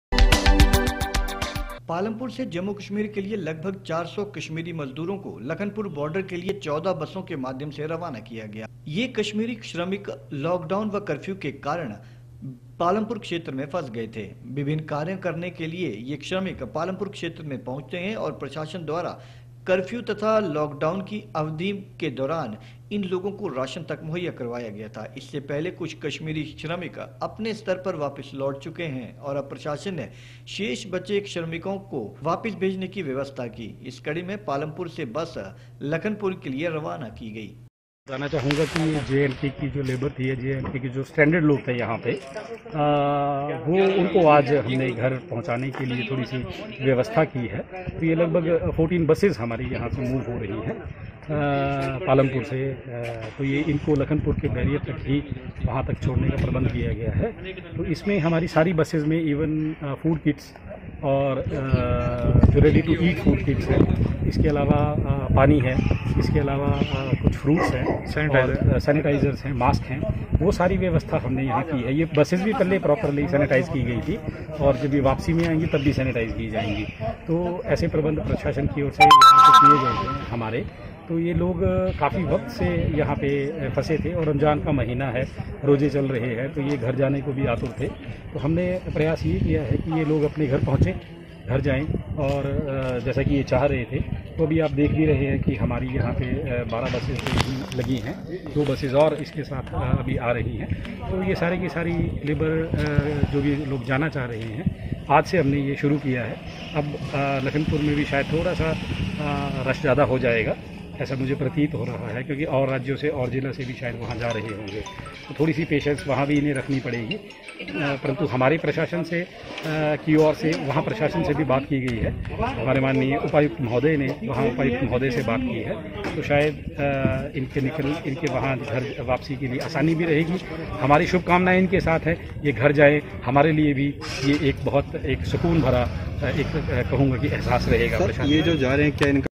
29 april 06 400 कश्मीरी मज़दूरों को लखनपुर बॉर्डर के लिये 14 बसों के माध्यम से रवाना किया गया
पालमपुर से जम्मू कश्मीर के लिये लगभग 400 कश्मीरी मज़दूरों को लखनपुर बॉर्डर के लिये 14 बसों के माध्यम से रवाना किया गया यह कश्मीरी श्रमिक का लॉक डाउन तथा कर्फ्यू के कारण क्षेत्र में ही फंसे हुए थे विभिन्न कार्य करने के लिए यह श्रमिक पालमपुर क्षेत्र में पहुंचते हैं प्रशासन द्वारा कर्फ्यू तथा ब्लॉक दाम की अवधि में इन लोगों को राशन उपलब्ध करवाया जा रहा था इससे पहले कुछ कश्मीरी श्रमिक अपने स्तर पर ही वापस लौट चुके हैं अब प्रशासन ने शेष बचे कश्मीरी श्रमिकों को वापस भेजने की व्यवस्था की है इसी कड़ी में पालमपुर से बसे लखनपुर के लिए रवाना हुई